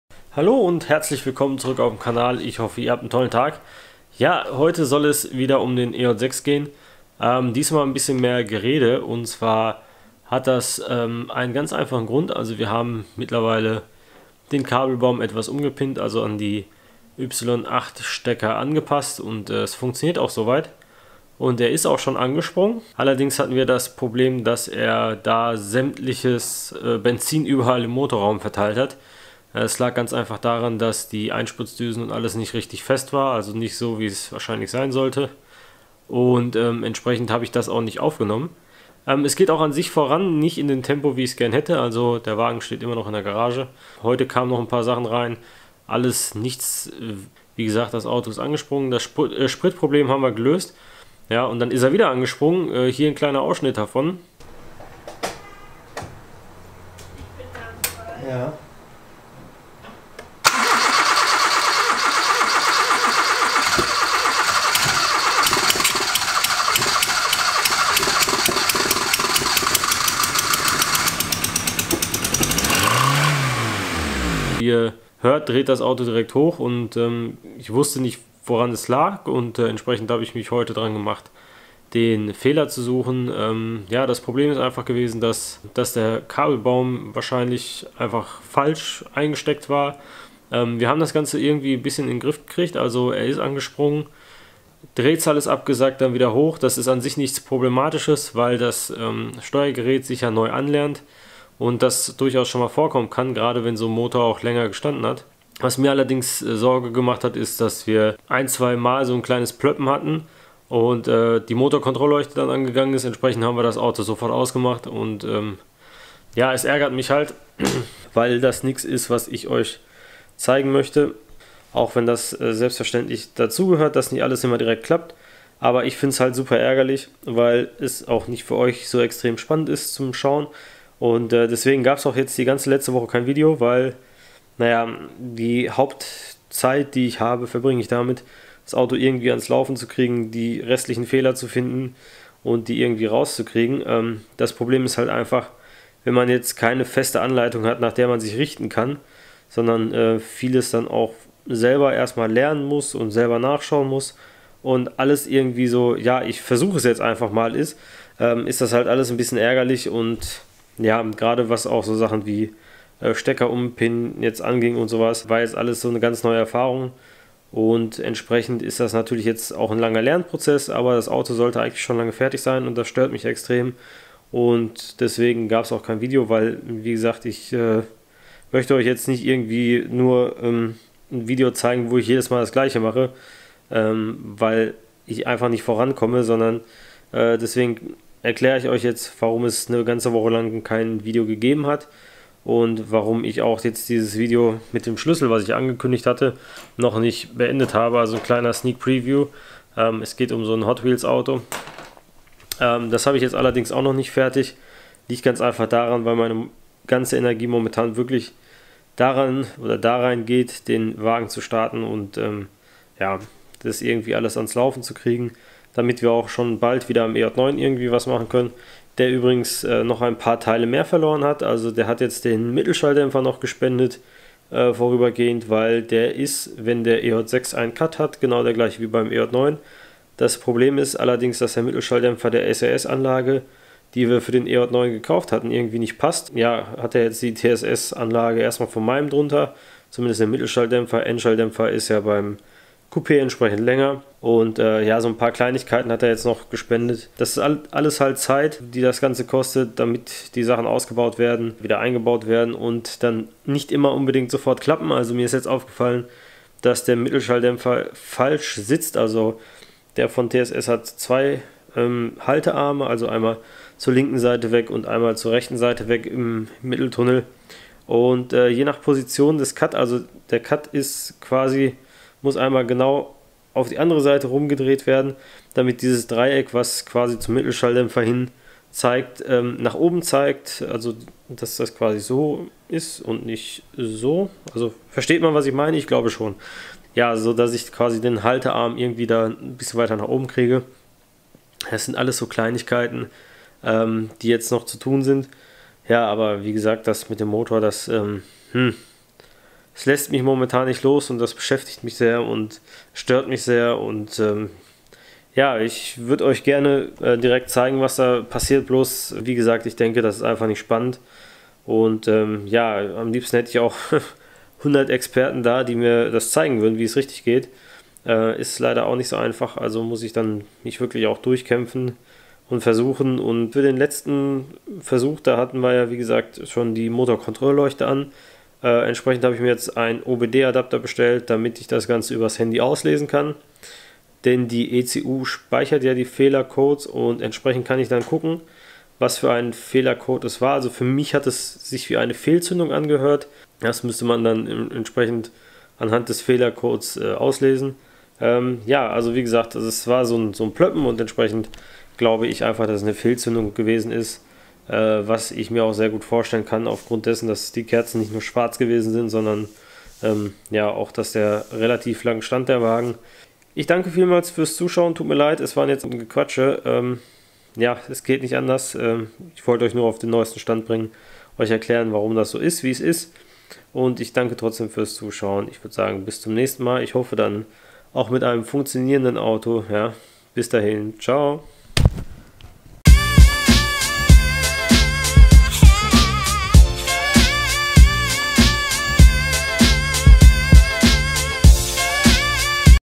Der EJ6 erwacht zum Leben ... Naja fast! Klartext!
Das Sorgenkind EJ6 will nicht so recht und ärgert mich damit ein wenig. Wieso ? schaut rein!

Garage Eight Logo by: Kimi Rogoz Design  (Instagram: Kimirogozdesign)

Kamera: Canon Eos 750D - Mehr nicht.